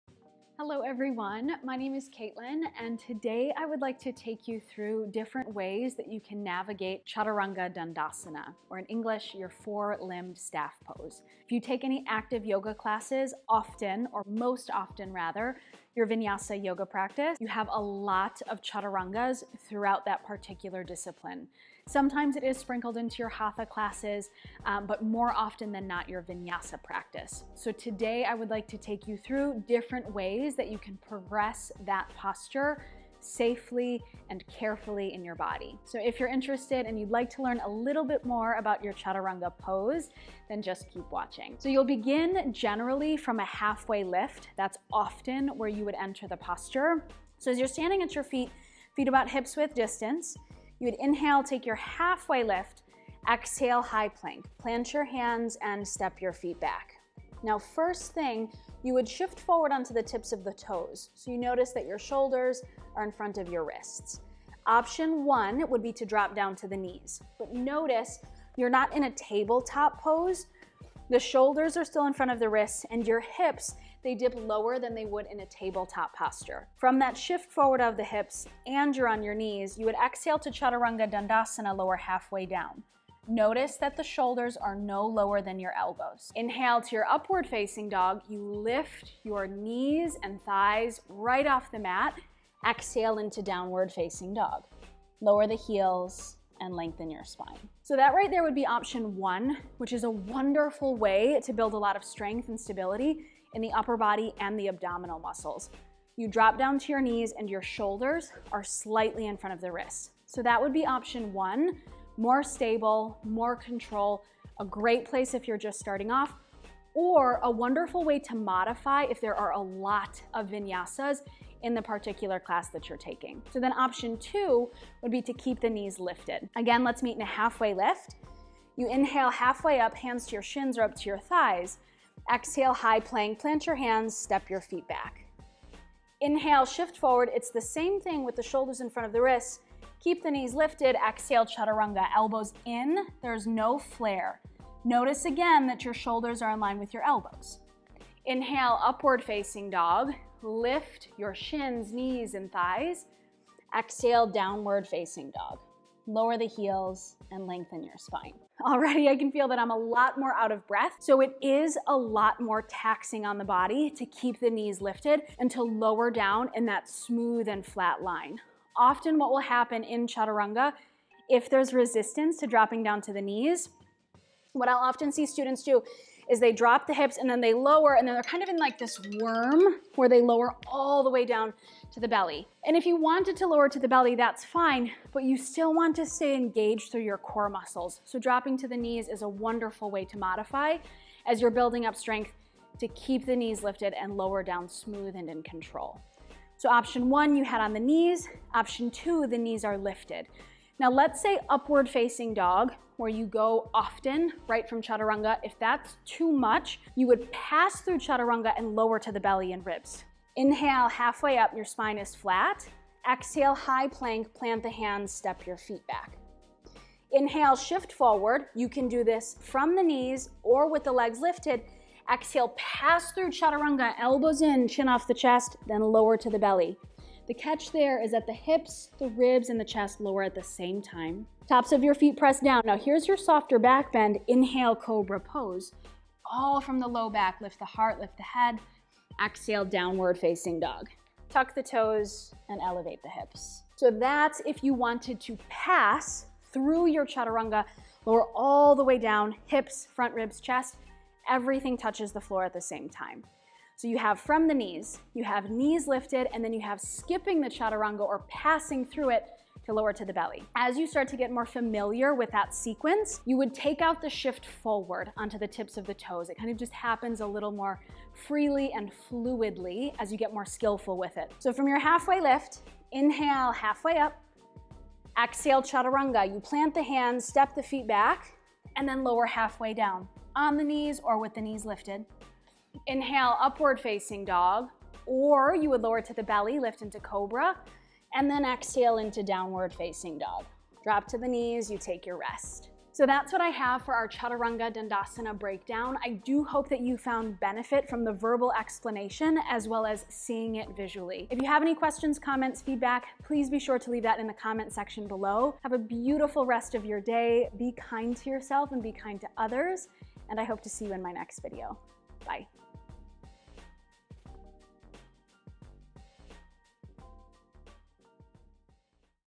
Chaturanga Tutorial for All Levels with Progressions
In this video I take your through four different Chaturanga Dandasana options depending on your wants & needs. Every time you take the pose it can look and feel so different depending on the given day and your energy levels. I hope that you find benefit!

xo

“Be kind to yourself and be kind to others.”